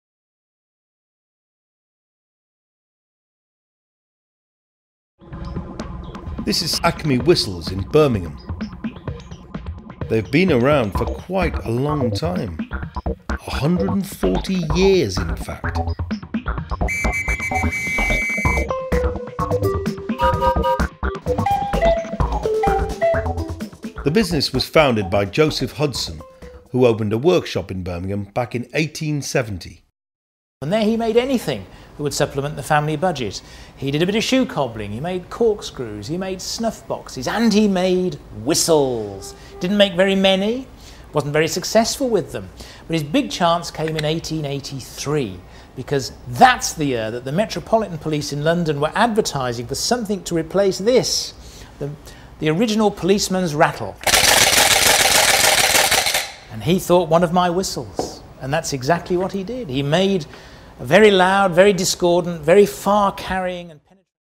CLIP 21 Costs & Working Capital At ACME Whistles
Covering its costs, fixed and variable, has long been a priority at Acme Whistles, Birmingham. But working capital and depreciation are key issues, too.